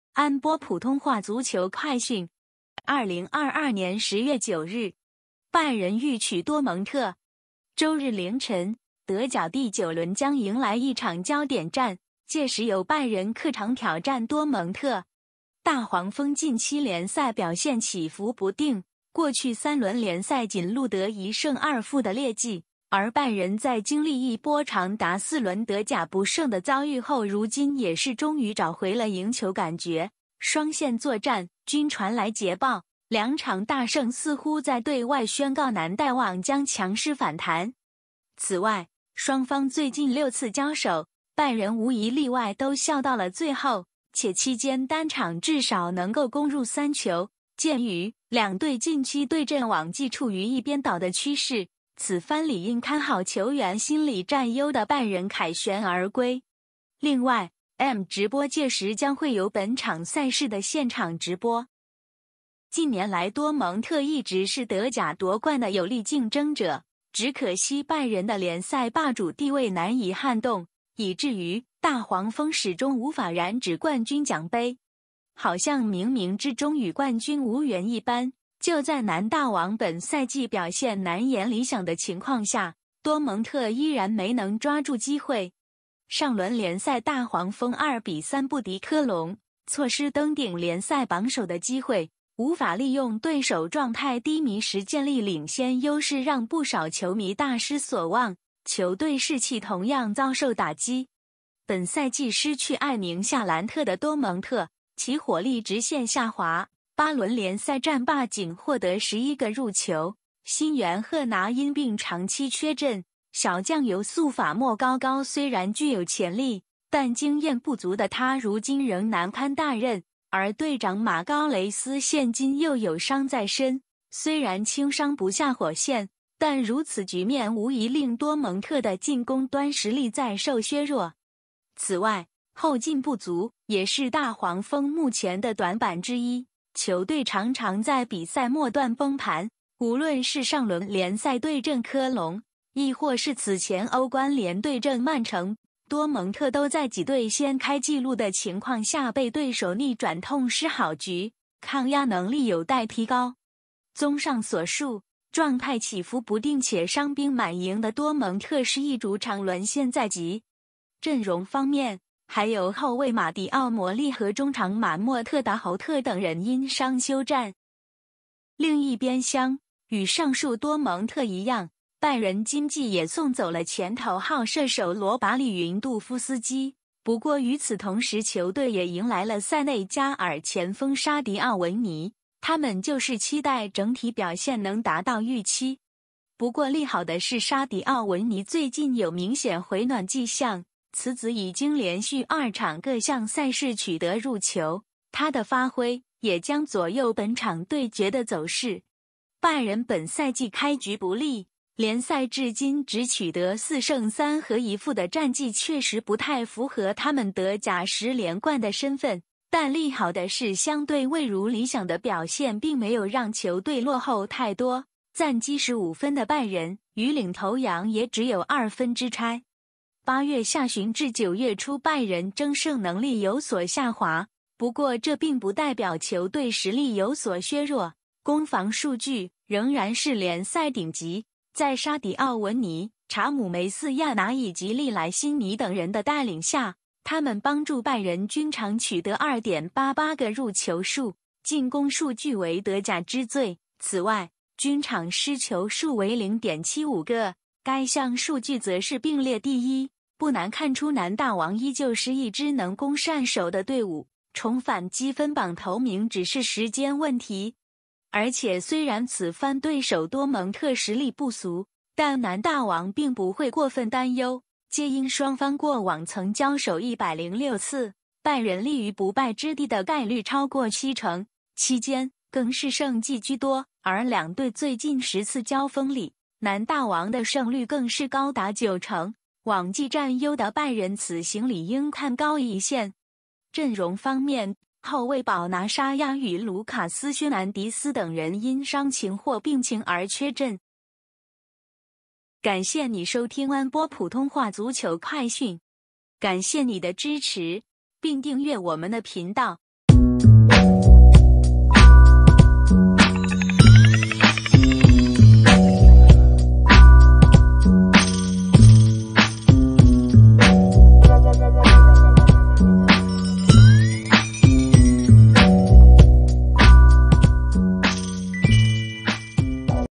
(普)【德甲-賽前分析】2022-10-09 多蒙特 VS 拜仁慕尼黑 | 拜仁欲取多蒙特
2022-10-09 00:30 拜仁慕尼黑 對
►單場賽事低至MOP$15, 立即下載M直播觀看 德甲 精彩賽事

週日凌晨，德甲第9輪將迎來一場焦點戰，屆時由拜仁客場挑戰多蒙特。大黃蜂近期聯賽表現起伏不定，過去3輪聯賽僅錄得1勝2負的劣績，而拜仁在經歷一波長達4輪德甲不勝的遭遇後如今也是終於找回了贏球感覺，雙線作戰均傳來捷報，兩場大勝似乎在對外宣告南大王將強勢反彈。此外，雙方最近6次交手，拜仁無一例外都笑到了最後，且期間單場至少能夠攻入3球，鑒於兩隊近期對陣往績處於一邊倒的趨勢，此番理應看好球員心理佔優的拜仁凱旋而歸。另外，M直播屆時將會有本場賽事的現場直播。

近年來多蒙特一直是德甲奪冠的有力競爭者，只可惜拜仁的聯賽霸主地位難以撼動，以致於大黃蜂始終無法染指冠軍獎盃。好像冥冥之中與冠軍無緣一般，就在南大王本賽季表現難言理想的情況下，多蒙特依然沒能抓住機會。上輪聯賽大黃蜂2比3不敵科隆，錯失登頂聯賽榜首的機會，無法利用對手狀態低迷時建立領先優勢讓不少球迷大失所望，球隊士氣同樣遭受打擊。本賽季失去艾寧.夏蘭特的多蒙特，其火力直線下滑，8輪聯賽戰罷僅獲得11個入球，新援賀拿因病長期缺陣，小將尤素法.莫高高雖然具有潛力，但經驗不足的他如今仍難堪大任，而隊長馬高.雷斯現今又有傷在身，雖然輕傷不下火線，但如此局面無疑令多蒙特的進攻端實力再受削弱。此外，後勁不足也是大黃蜂目前的短板之一，球隊常常在比賽末段崩盤，無論是上輪聯賽對陣科隆，抑或是此前歐冠聯對陣曼城，多蒙特都在己隊先開紀錄的情況下被對手逆轉痛失好局，抗壓能力有待提高。綜上所述，狀態起伏不定且傷兵滿營的多蒙特是役主場淪陷在即。陣容方面，還有後衛馬迪奧.摩利和中場馬莫特.達侯特等人因傷休戰。

另一邊廂，與上述多蒙特一樣，拜仁今季也送走了前頭號射手羅拔.利雲度夫斯基，不過與此同時球隊也迎來了塞內加爾前鋒沙迪奧.文尼，他們就是期待整體表現能達到預期。不過利好的是沙迪奧.文尼最近有明顯回暖跡象，此子已經連續2場各項賽事取得入球，他的發揮也將左右本場對決的走勢。拜仁本賽季開局不利，聯賽至今只取得4勝3和1負的戰績確實不太符合他們德甲十連冠的身份，但利好的是相對未如理想的表現並沒有讓球隊落後太多，暫積15分的拜仁與領頭羊也只有2分之差。8月下旬至9月初拜仁爭勝能力有所下滑，不過這並不代表球隊實力有所削弱，攻防數據仍然是聯賽頂級，在沙迪奧.文尼、查姆.梅斯亞拿以及利萊.辛尼等人的帶領下，他們幫助拜仁均場取得2.88個入球數，進攻數據為德甲之最；此外，均場失球數為0.75個，該項數據則是並列第一，不難看出南大王依舊是一支能攻善守的隊伍，重返積分榜頭名只是時間問題。而且雖然此番對手多蒙特實力不俗，但南大王並不會過分擔憂，皆因雙方過往曾交手106次，拜仁立於不敗之地的概率超過七成，期間更是勝績居多；而兩隊最近10次交鋒裡，南大王的勝率更是高達九成，往績佔優的拜仁此行理應看高一線。陣容方面，後衛保拿.沙亞與盧卡斯.靴南迪斯等人因各種原因而缺陣。